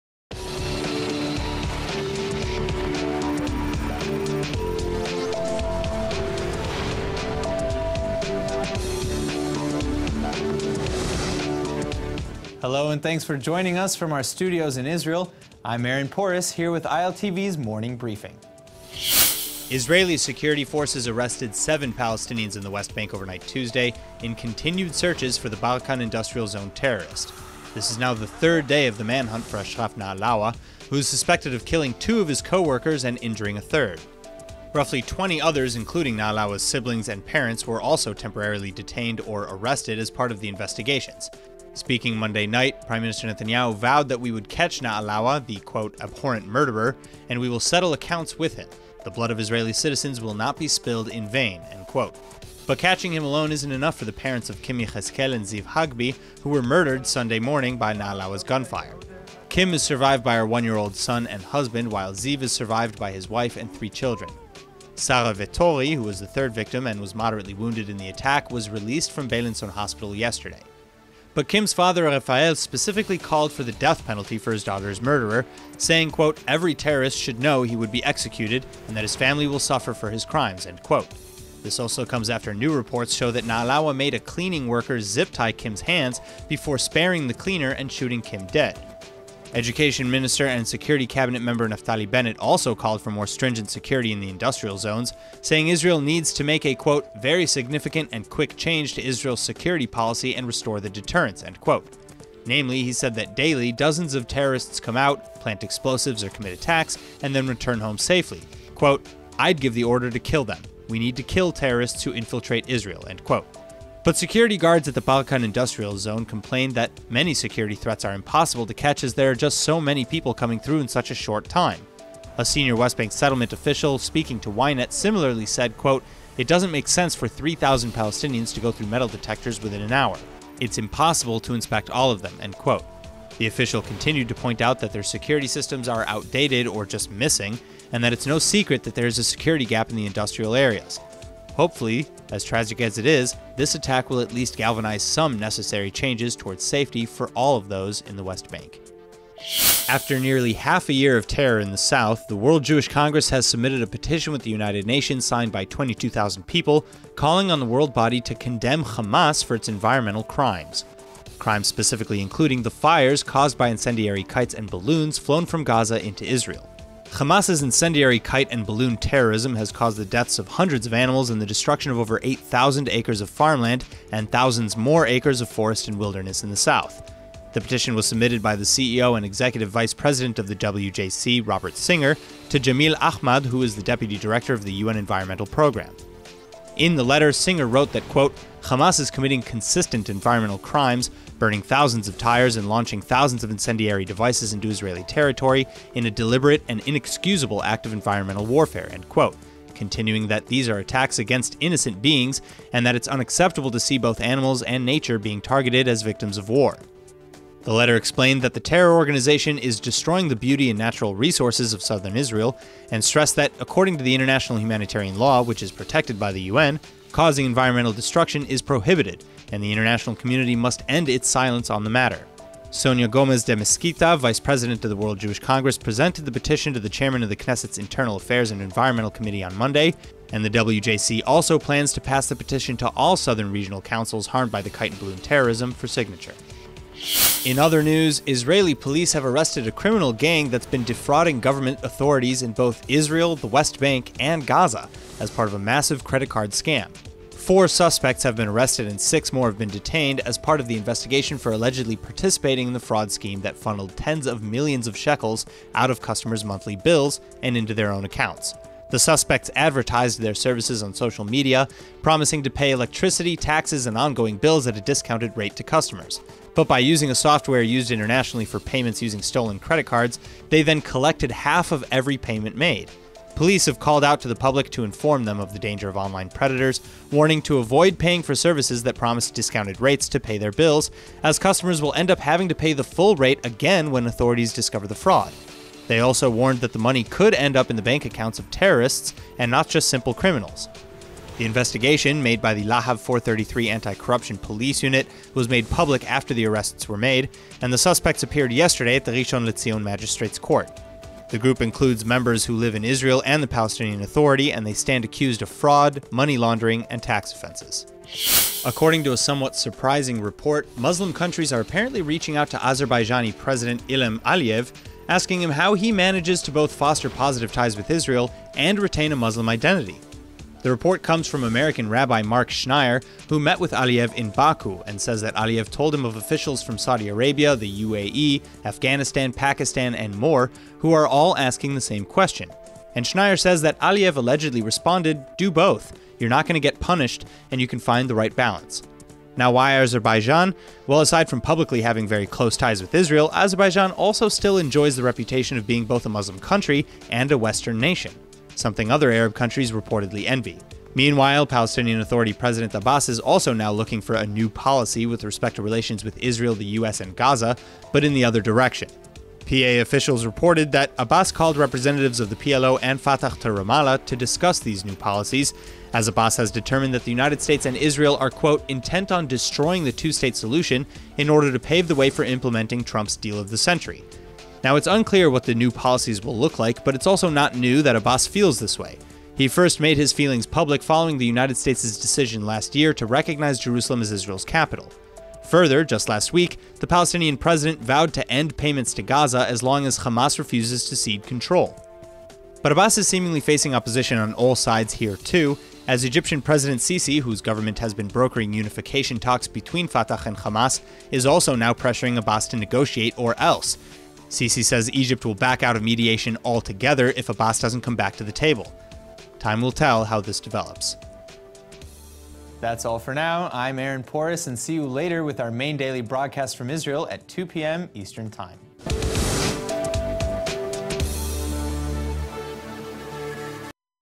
Your Morning News From Israel - Oct. 09, 2018.
BARKAN TERRORIST MANHUNT CONTINUES INTO 3RD DAY, W.J.C. PETITIONS AGAINST HAMAS IN THE U.N., CRIMINAL GANG ARRESTED; CHARGED WITH FRAUD, AND ISLAMIC COUNTRIES TALK RELATIONS WITH ISRAEL.